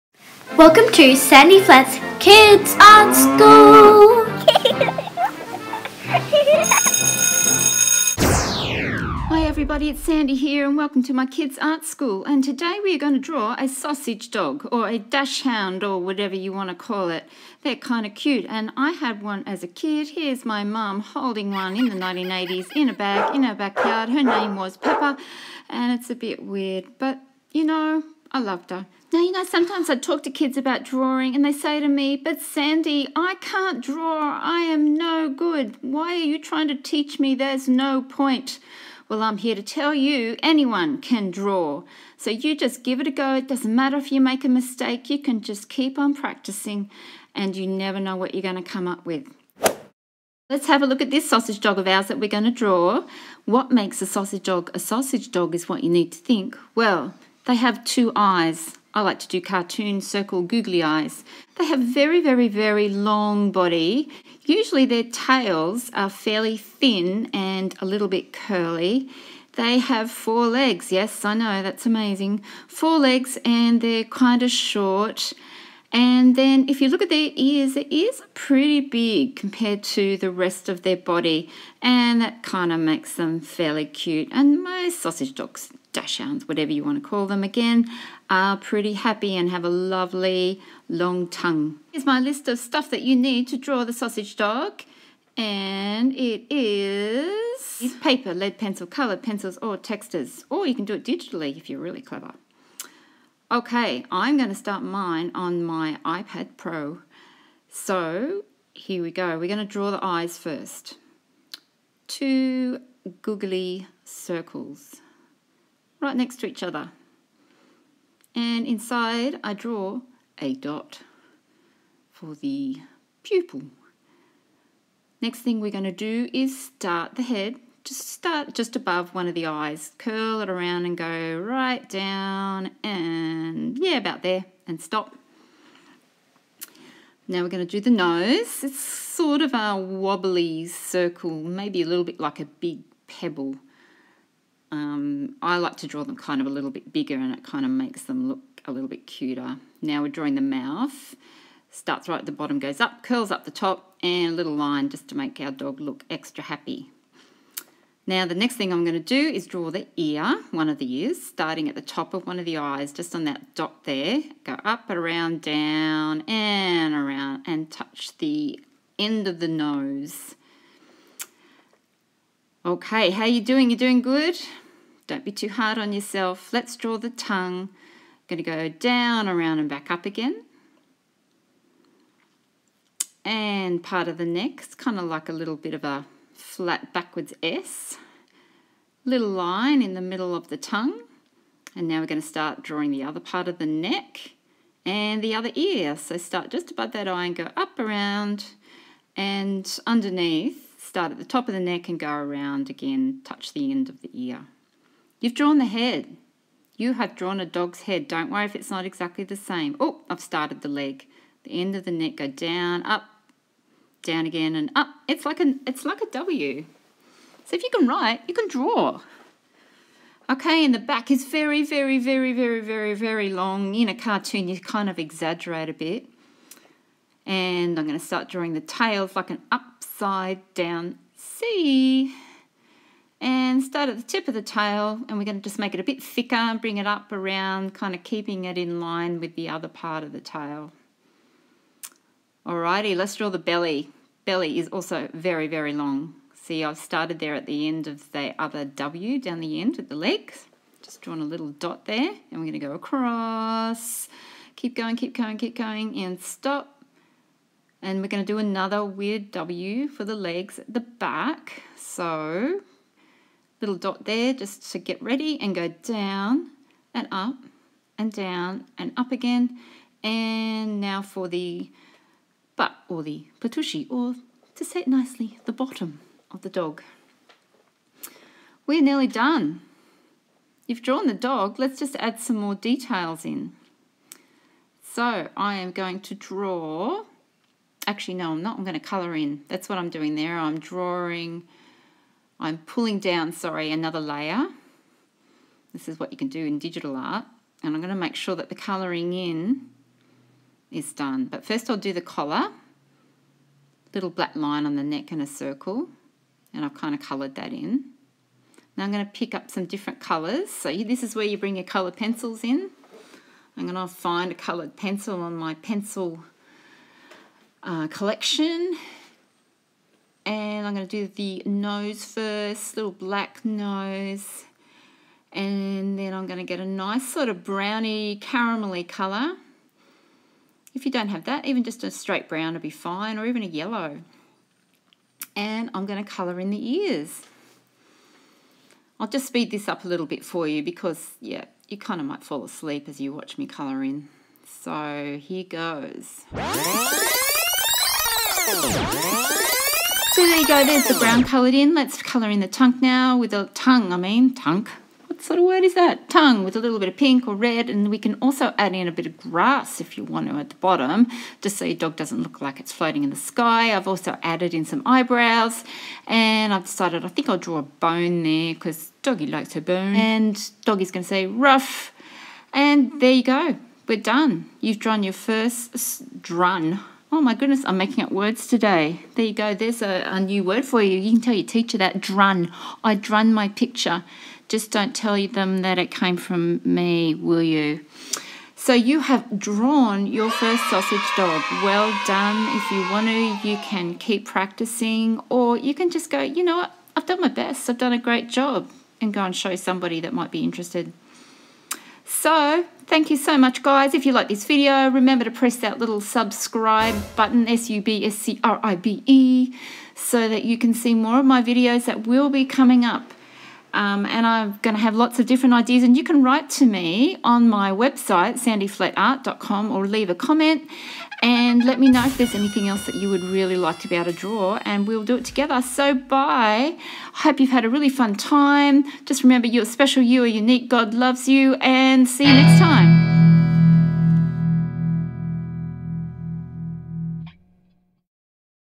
Kids Art School - How to draw a sausage dog - dachshund
A fun kids art lesson on how to draw sausage dogs - dachshunds